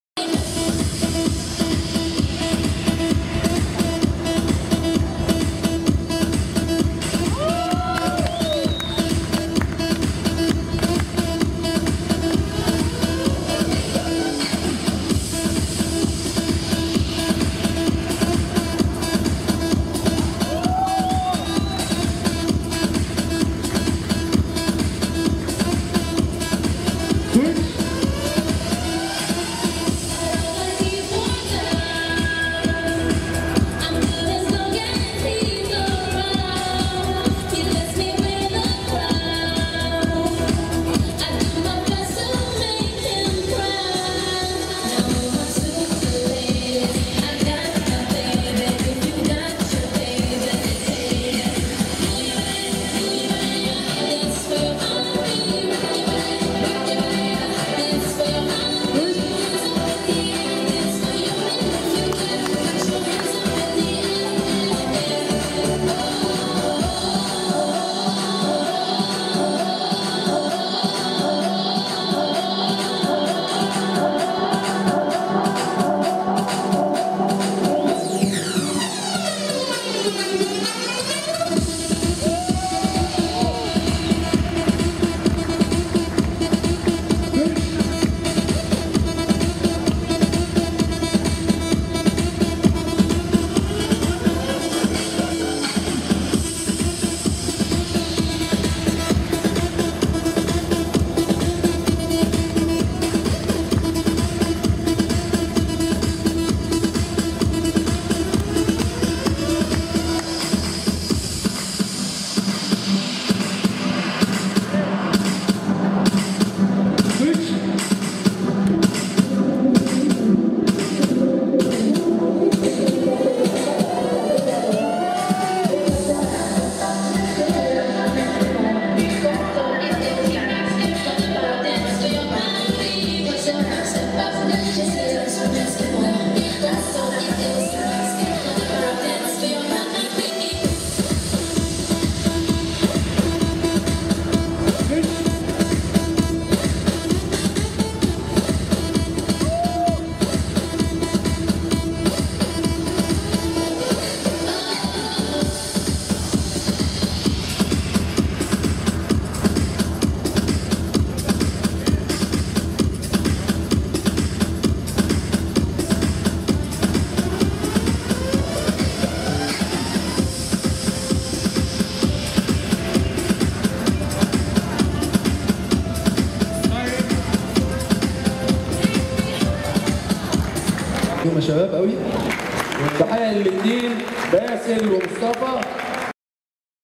Disha vs Beso | H&S Egypt Semi Final
H&S Semi finall battle number one
Disha vs Beso
Judges panel was: Ahmed Fathy , Khaled Bebo , Hazem Emam and Yorok